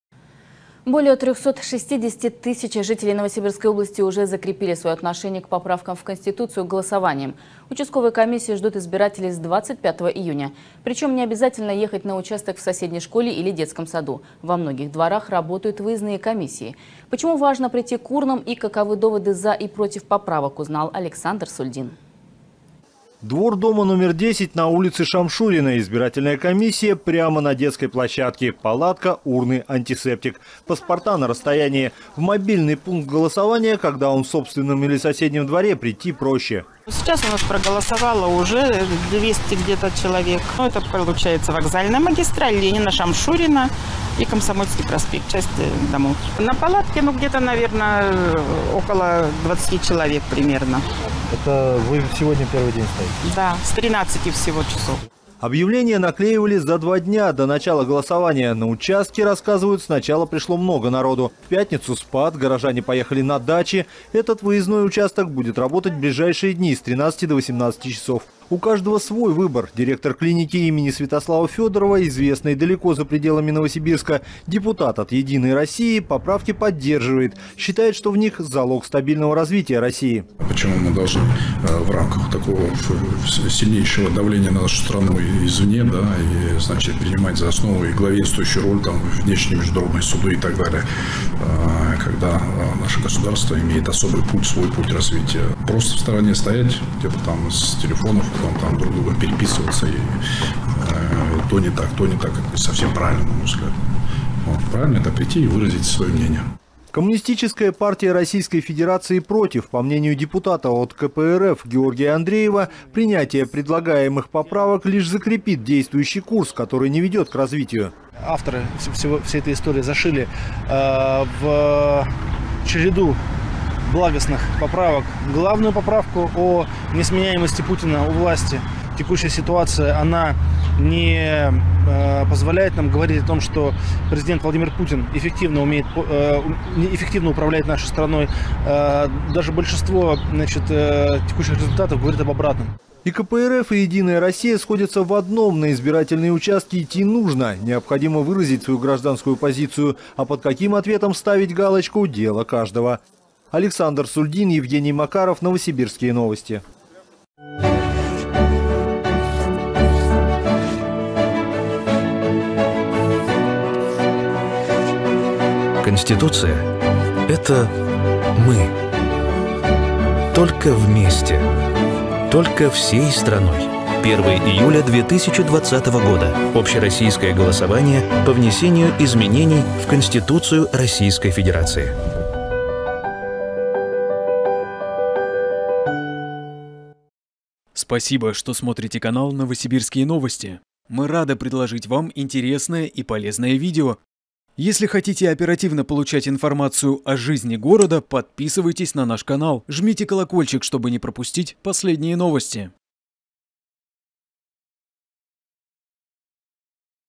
360 000 новосибирцев проголосовали по поправкам в Конституцию
Более 360 000 жителей Новосибирской области уже закрепили своё отношение к поправкам в Конституцию голосованием. Участковые комиссии ждут избирателей с 25 июня. Причём не обязательно ехать на участок в соседней школе или детском саду. Во многих дворах работают выездные комиссии. Почему важно прийти к урнам и каковы доводы «за» и «против» поправок, узнали «Новосибирские новости».